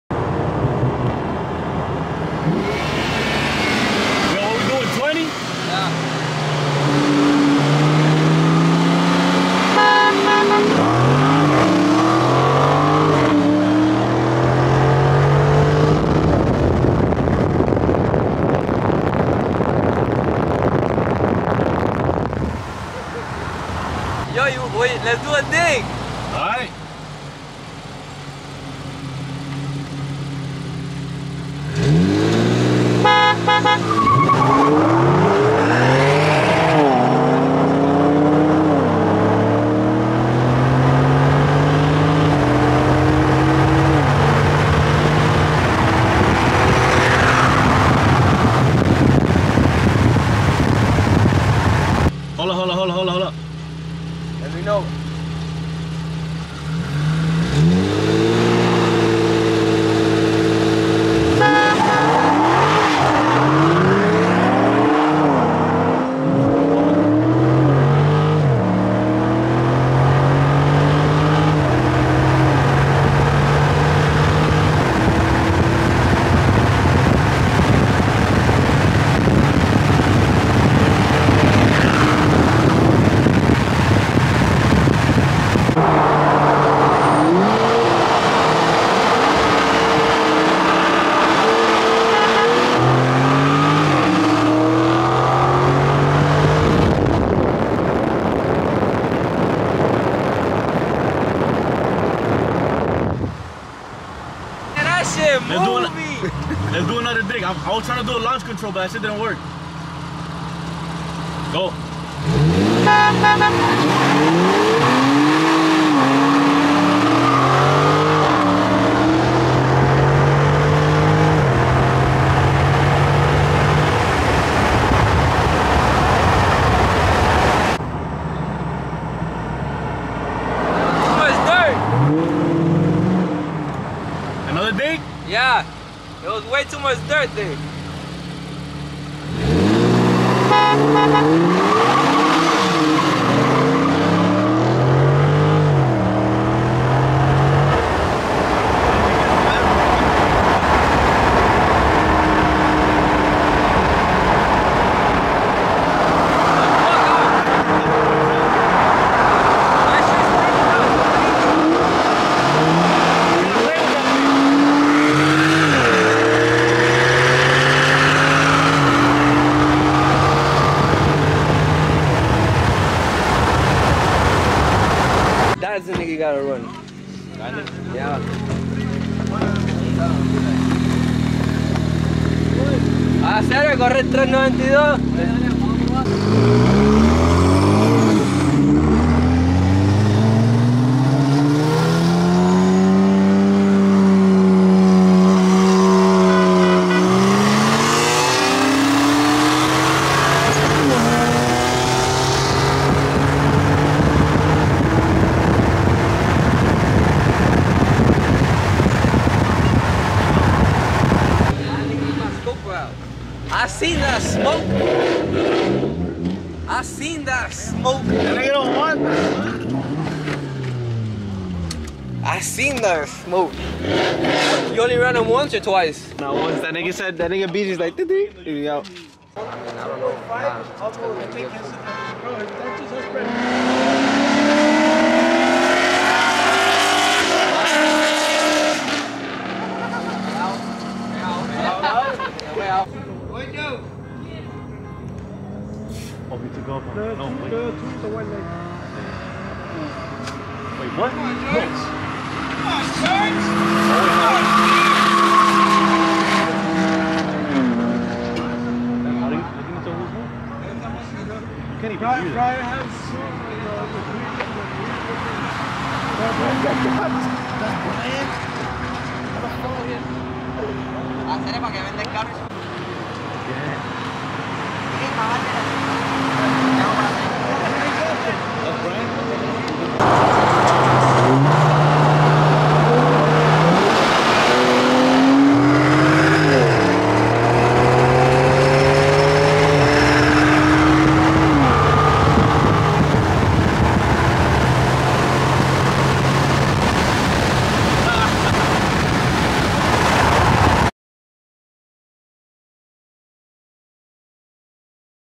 SCAT PACK CHARGER VS Q50 | MIDNIGHT RUNS
SCAT PACK CHARGER VS Q50 in midnight runs also ran against a fully modded BMW

About YvngYatted:
AYE AYE AYE It's YvngYatted